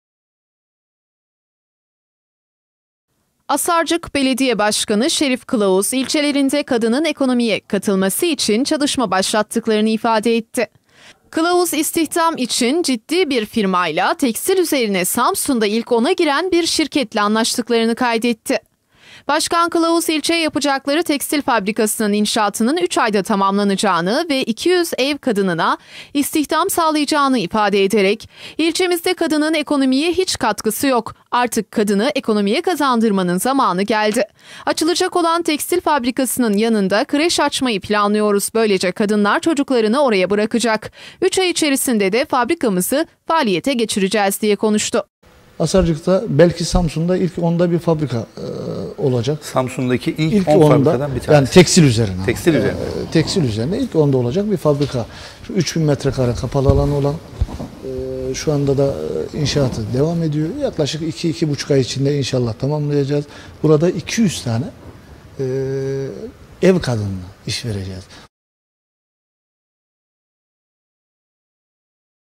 ASARCIK'TA KADIN İŞ DÜYASINA GİRİYOR
Asarcık Belediye Başkanı Şerif Kılağuz, ilçelerinde kadının ekonomiye katılımını sağlayarak 200 ev kadınına istihdam sağlayacaklarını söyledi.